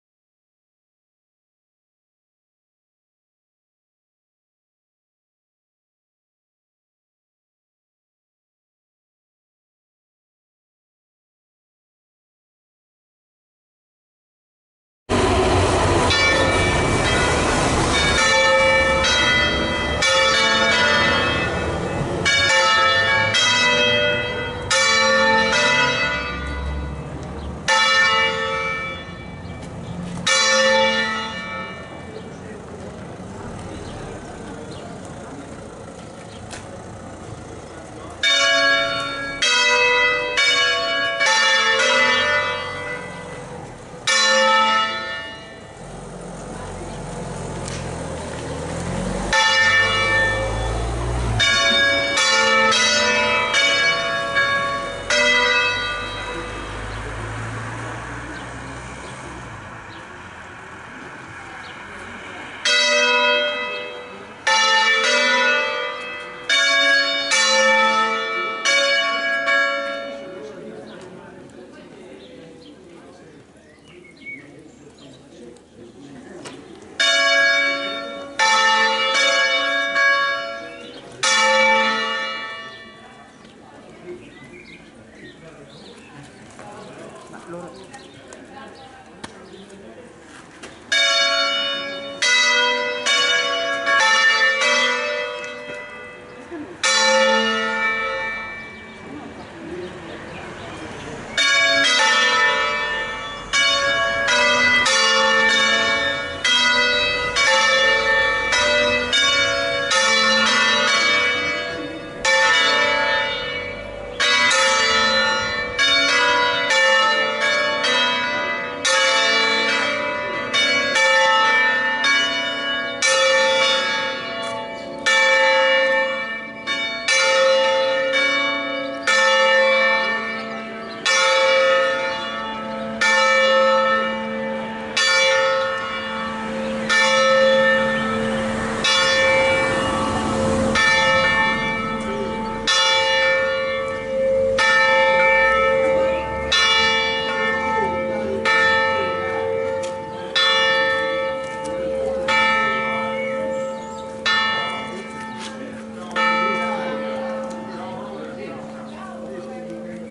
Le campane di Rivergaro (PC) - Fraz. Ottavello - Chiesa Parrocchiale di San Bartolomeo Apostolo (1)
Rivergaro (PC) - Fraz. Ottavello
Chiesa Parrocchiale di San Bartolomeo Apostolo
Diocesi di Piacenza-Bobbio
Vicariato della Bassa Val Trebbia e della Val Lauretta
Concerto di 3 campane in LA3
Concerto solenne a 3 campane eseguito appositamente per la ripresa per la fine della Santa Messa Festiva delle ore 9.00 nella Solennità del Corpus Domini

Domenica 11 Giugno 2023, ore 9.55

Ringrazio tutti i presenti per la grande disponibilità